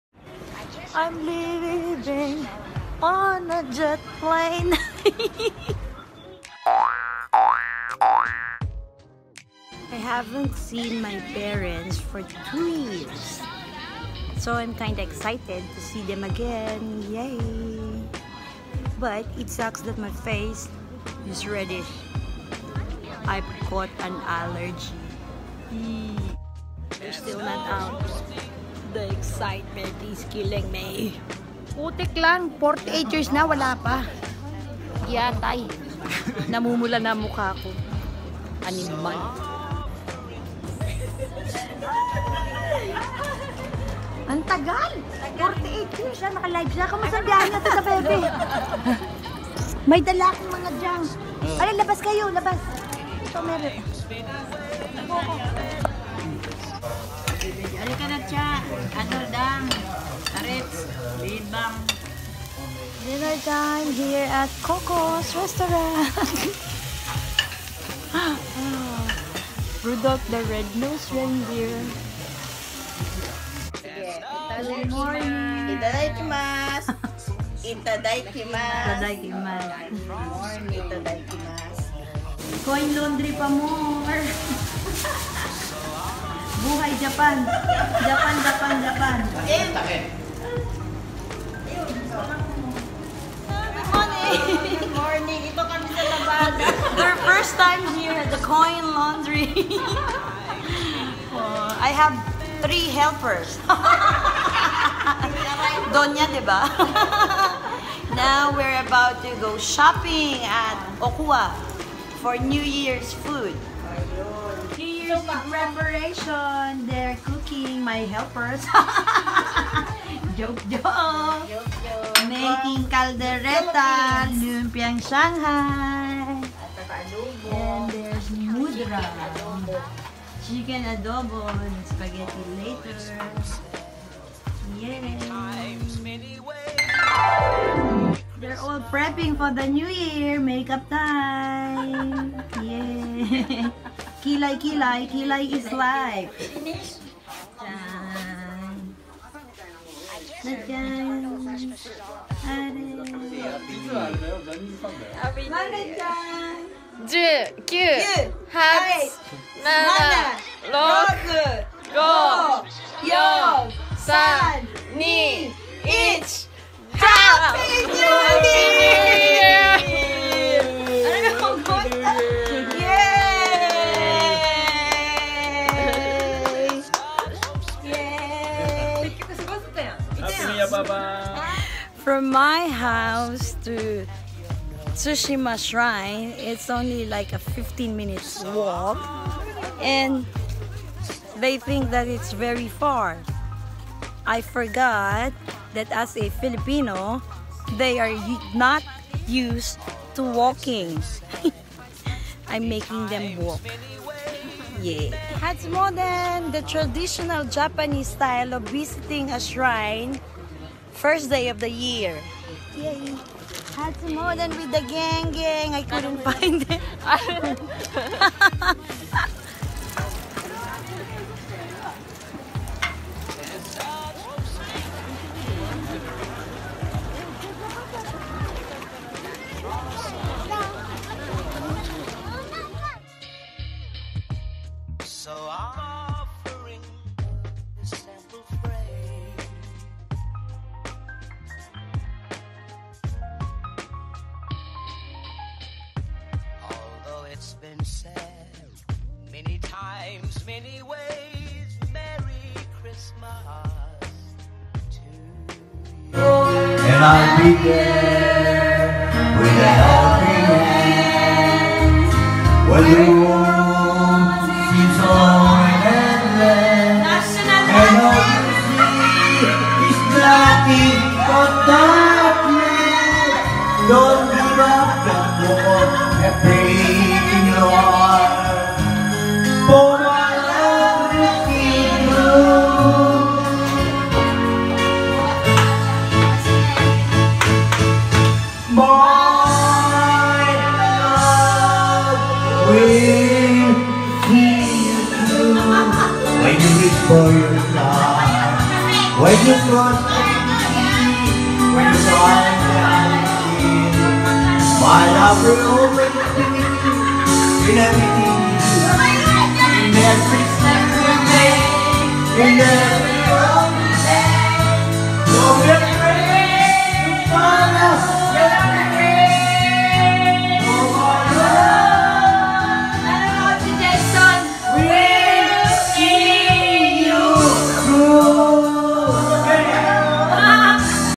The Tourists Vlog #028
Parents and Aunt first time here in JapaLandia😀